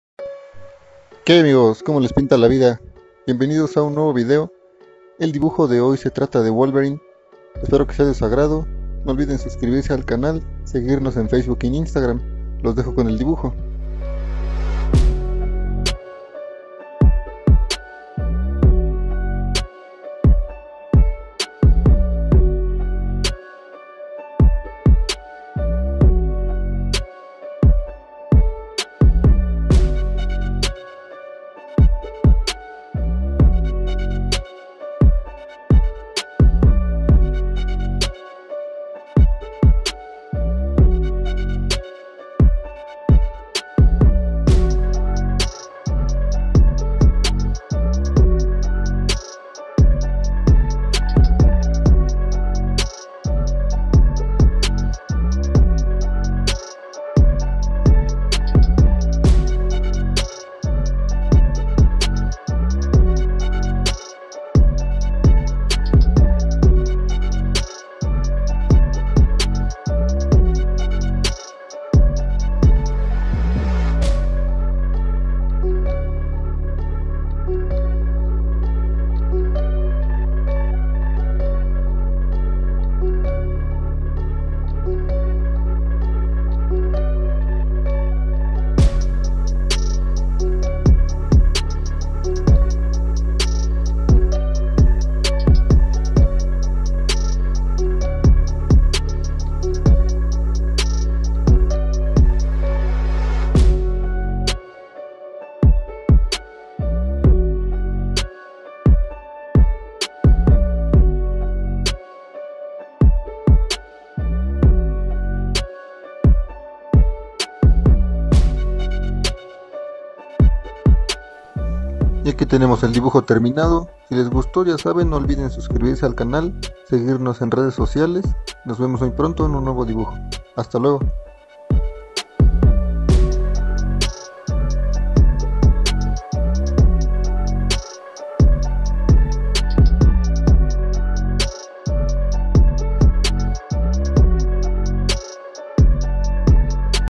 Dibujando a Wolverine (Logan) de X-MEN - Speed Drawing - Wolverine from X-MEN -#90
Dibujo de Logan (wolverine) de los X-Men hecho a bolígrafo, gracias por mirar el vídeo :)